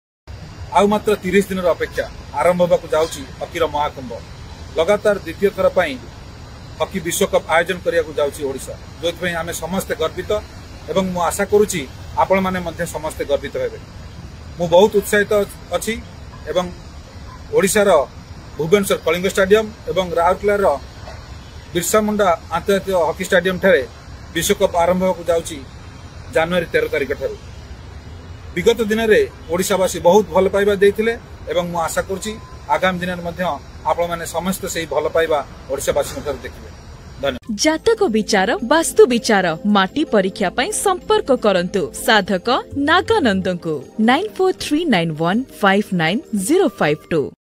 ୩୦ ଦିନ ପରେ ଓଡ଼ିଶାର ରାଜଧାନୀ ଭୁବନେଶ୍ବର ଏବଂ ଇସ୍ପାତ ସହର ରାଉରକେଲାରେ ଆରମ୍ଭ ହେବାକୁ ଯାଉଛି ହକି ବିଶ୍ୱକପ...!!
ଆଉ ମାତ୍ର ୩୦ ଦିନ ପରେ ଓଡ଼ିଶାର ରାଜଧାନୀ ଭୁବନେଶ୍ବର ଏବଂ ଇସ୍ପାତ ସହର ରାଉରକେଲାରେ ଆରମ୍ଭ ହେବାକୁ ଯାଉଛି ହକି ବିଶ୍ୱକପ। ଏନେଇ ନିଜର ଖୁସି ଜାହିର୍ କରି ରାଜ୍ୟବାସୀଙ୍କ ଉଦ୍ଦେଶ୍ୟରେ ବାର୍ତ୍ତା ରଖୁଛନ୍ତି ମାନ୍ୟବର କ୍ରୀଡ଼ାମନ୍ତ୍ରୀ ଶ୍ରୀ ତୁଷାରକାନ୍ତି ବେହେରା ।